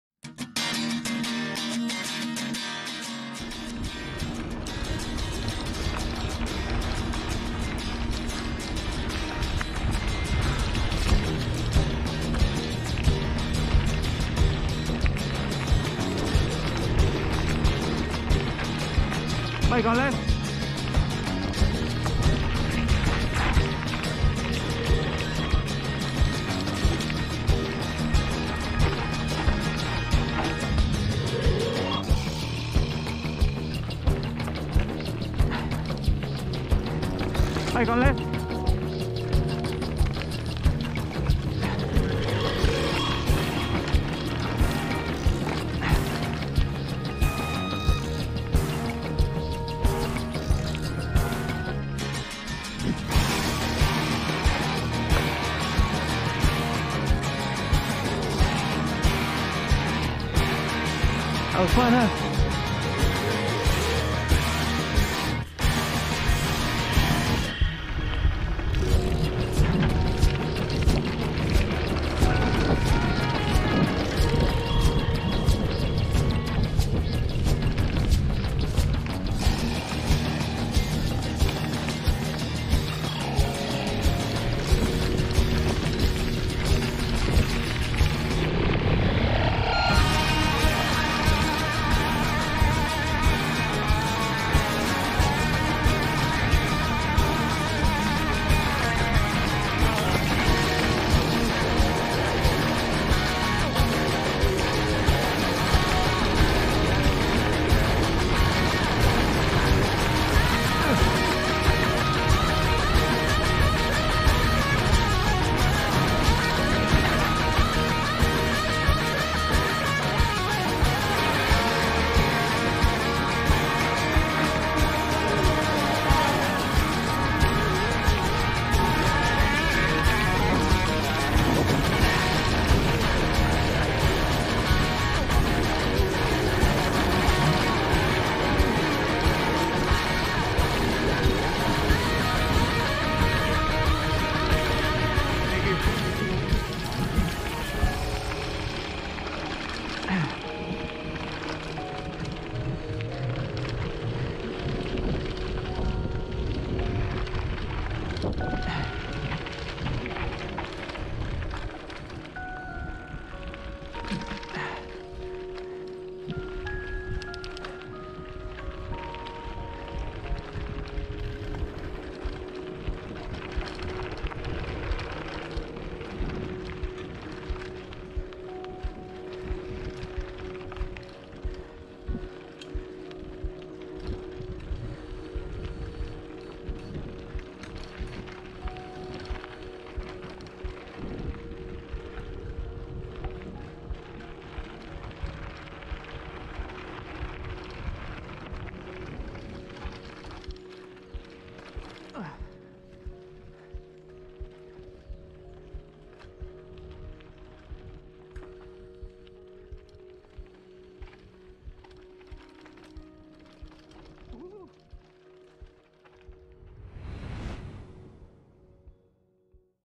MTB through Rancho Cañada del Oro
Awesome ride for mountain biking and hiking at Rancho Cañada del Oro in Morgan Hill CA. In this occasion with hit it with the bikes.